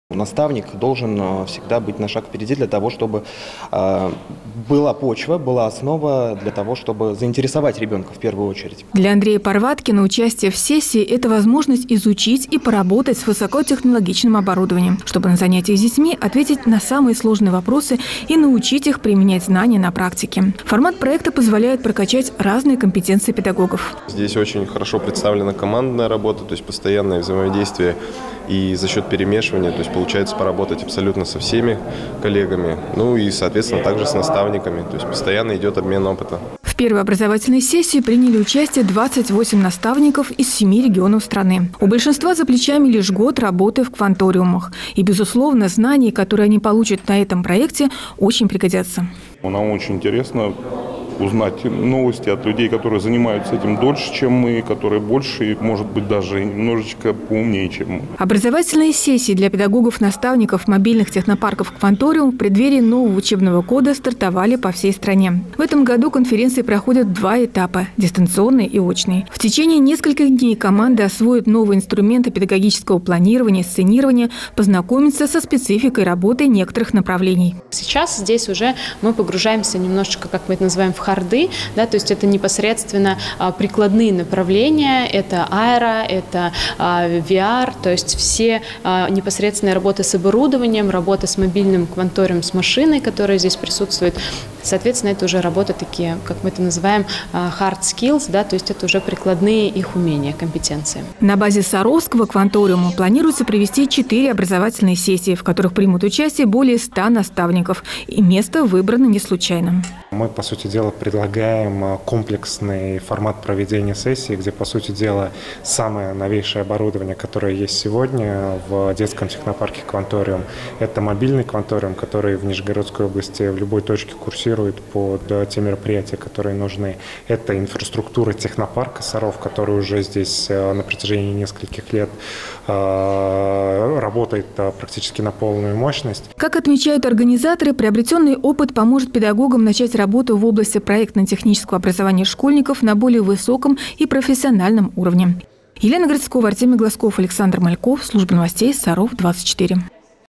Повышение квалификации
В детском технопарке «Кванториум Саров» проходят образовательные сессии для педагогов-наставников.